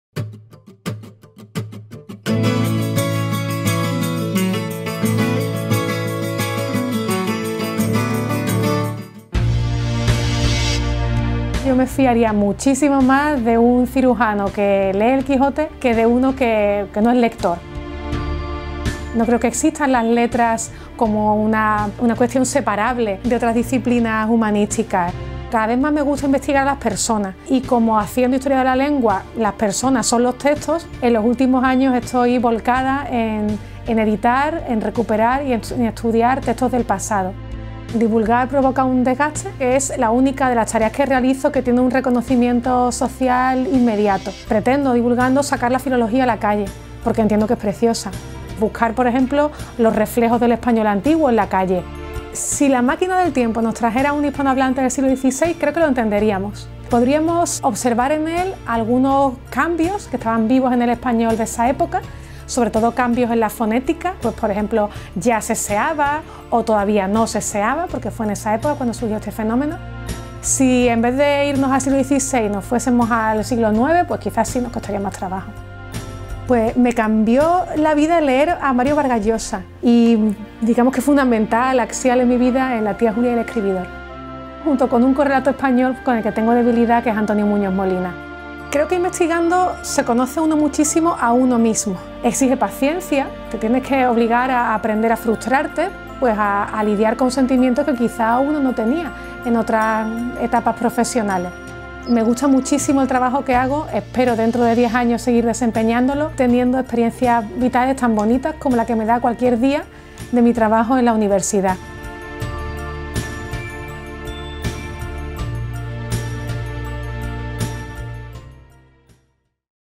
AS Lola Pons, the philologist delving into the history of Spanish to bring it to the masses
A specialist in the history of Spanish, who reports regularly in the press, on the radio, on television and on social media, this professor from the University of Seville has one aim: to make the general public enjoy philology as much as she does in her everydaywork. Why do some Andalusians lisp and others don’t? Are there still traces remaining today of Old Spanish? How did Spanish people speak in the 16th Century? Lola Pons explores questions like these in order to pass on the answers, in an entertaining and enjoyable way, to all those who wish to get involved in the adventure of learning about the history of the Spanish language.